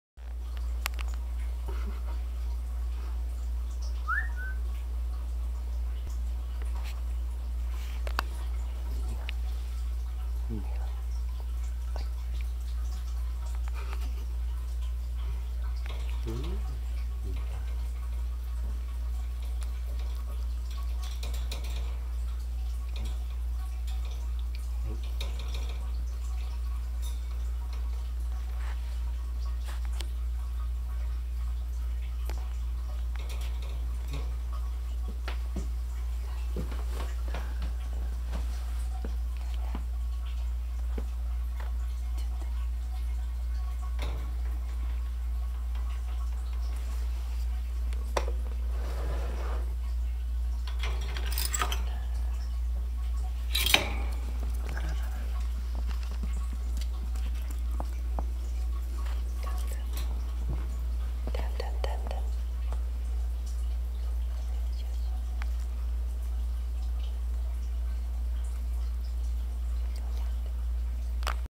６月１日生まれのトイプードル ブラックタン
甘えん坊の男の子です。